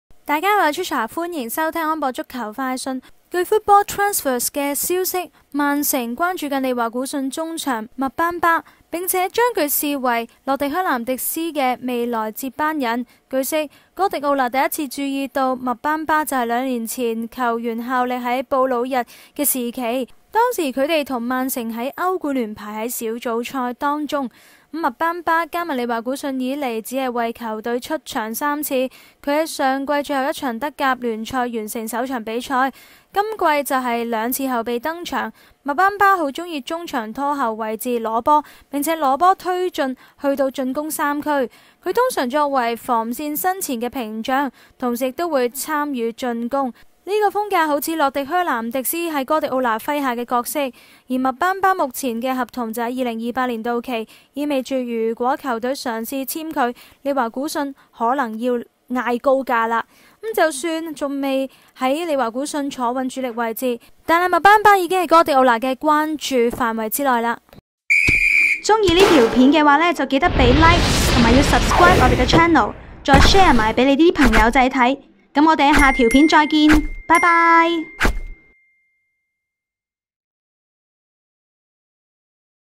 【英超消息】2023-10-26 曼城瞄準麥班巴 [聲音報導: Trisha]
英超 據FootballTransfers的消息，曼城 在關注利華古遜中場諾亞.麥班巴 ，並且將他視為洛迪.靴南迪斯在未來的接班人。

據悉，哥迪奧拿第一次注意到諾亞.麥班巴是在兩年前球員效力於布魯日時期與曼城的歐冠聯小組賽中。

諾亞.麥班巴加盟利華古遜以來僅僅為球隊登場3次，他在上賽季最後一場德甲聯賽完成首秀，本賽季則是兩次後備登場。

諾亞.麥班巴喜歡在中場的拖後位置持球並且持球推進到進攻三區，他通常作為防線身前的屏障，同時也會參與進攻，這很像洛迪.靴南迪斯在哥迪奧拿麾下的角色。

諾亞.麥班巴目前的合同在2028年到期，這意味著如果有球隊嘗試簽下他，利華古遜可能索要高價。

儘管還沒有在利華古遜坐穩主力位置，但諾亞.麥班巴已經在哥迪奧拿的關注範圍內。